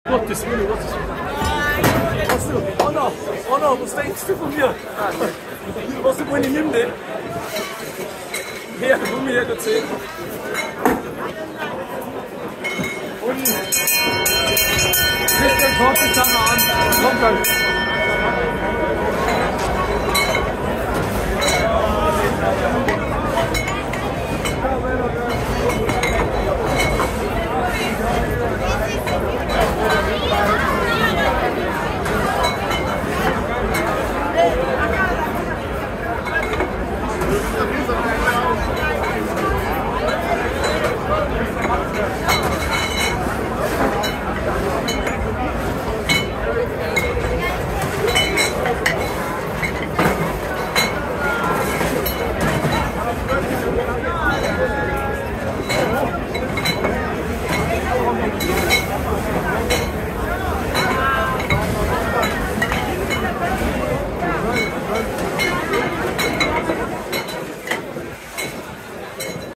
10 mal Sonne ins Glas ☀️🫶 Der Augustiner Holzfassausschank macht's möglich💪
Auf da Wiesn ausm Hirschn! Die Schänken in der Augustiner Festhalle 💪 Profis am Werk👌